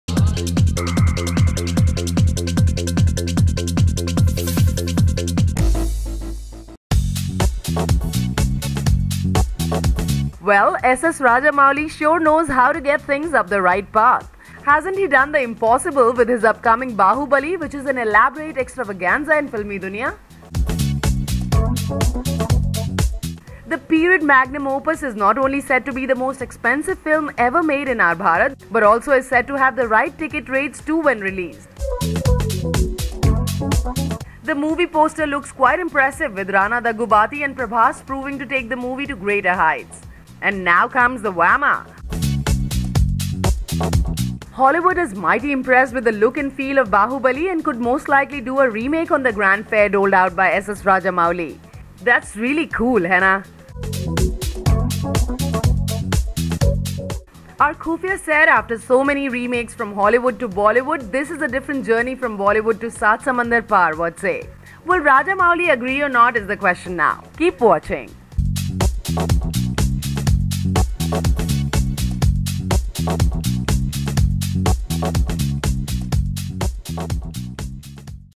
Baahubali To Be Remade In Hollywood
Well SS Rajamouli sure knows how to get things up the right path. Hasn’t he done the impossible with his upcoming Bahubali which is an elaborate extravaganza in filmi duniya?

The period magnum opus is not only said to be the most expensive film ever made in our Bharat but also is said to have right ticket rates too when released.

The movie poster looks quite impressive with Rana Daggubati and Prabas proving to take the movie to greater heights.
And now comes the whammer! Hollywood is mighty impressed with the look and the feel of Bahubali and could most likely do a remake on the grand fare doled out by SS Rajamouli.

That’s real cool hai na? Our khoofiya said that after so many remakes from Hollywood to Bollywood this is a different journey from Bollywood to saat samundar paar what say?
Will Rajamouli agree or not is the question now. Keep watching.